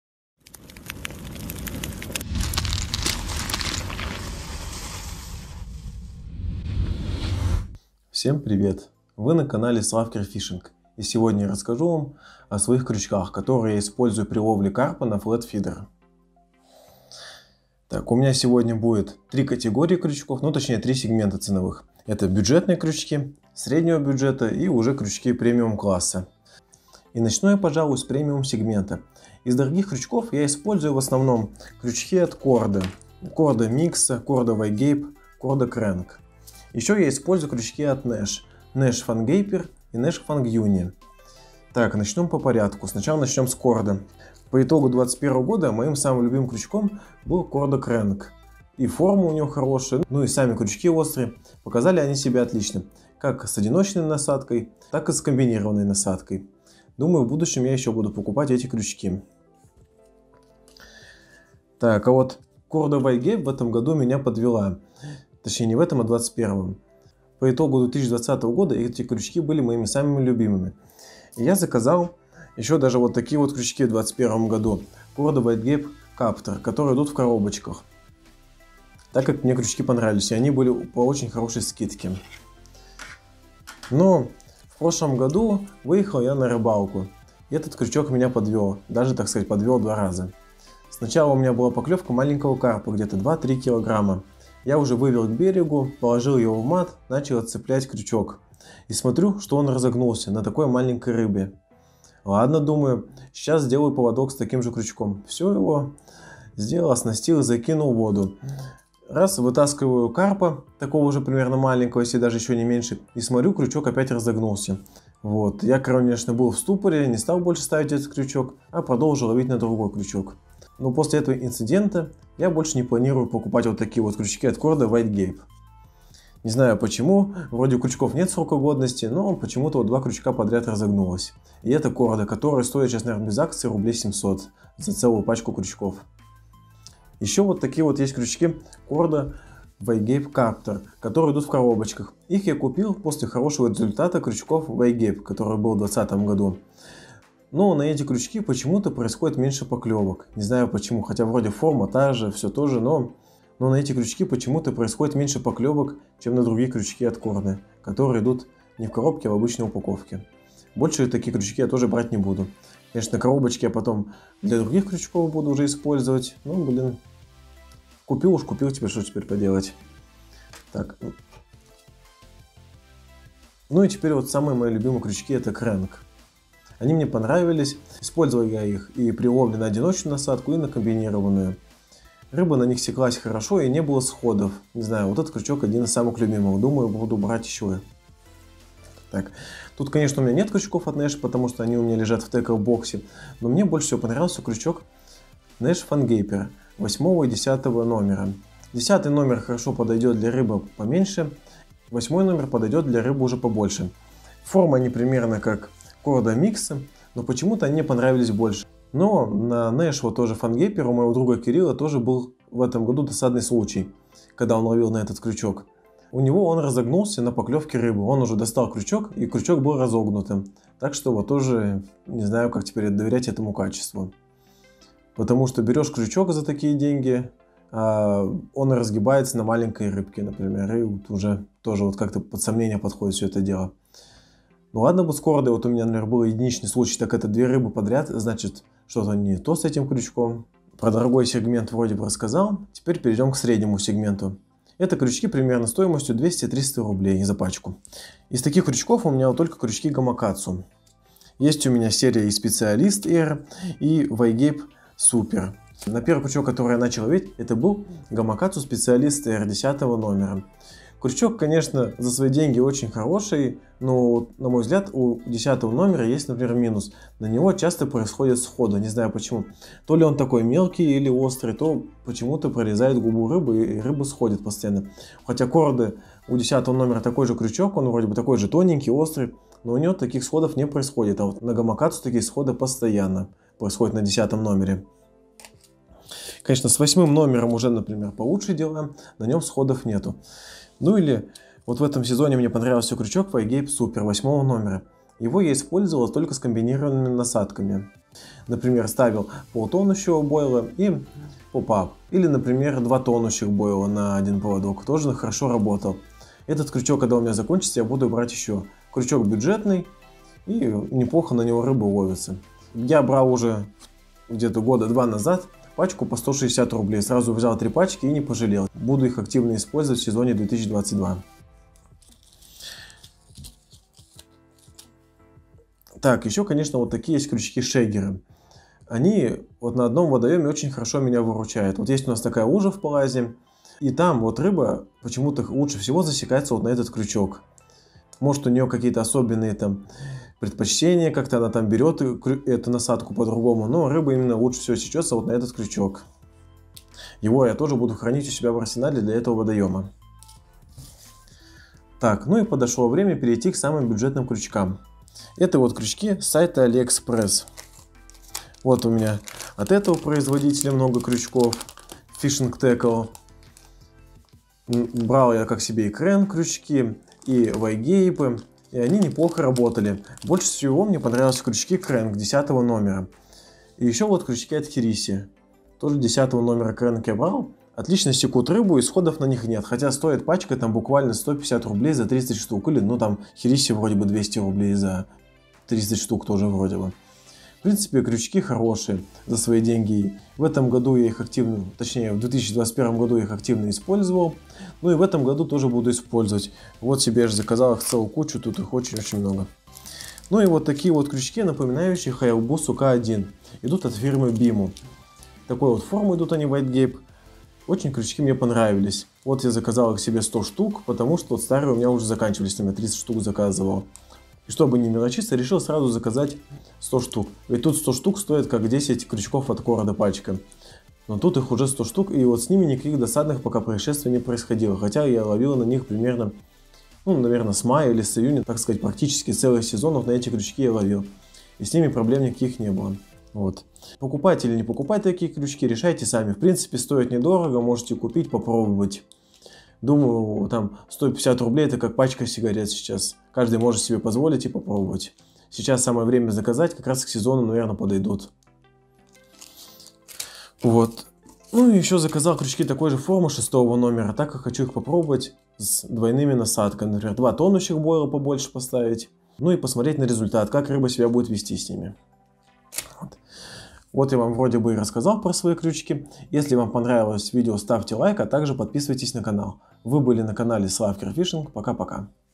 Крючки для ФЛЭТ ФИДЕРА Korda Nash Gamakatsu Китай
Всем привет) в этом видео  я расскажу Вам о своих крючках, которые я использую при ловле карпа на флэт фидер)
В видео Вы найдёте крючки разных ценовых категорий: Korda Nash Gamakatsu, а также крючки с Алиэкспресс.